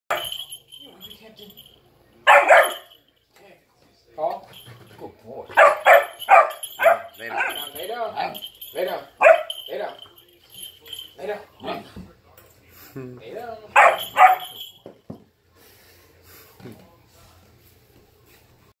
Training German Shepherd puppy to lay down argues back
Training German Shepherd puppy to lay down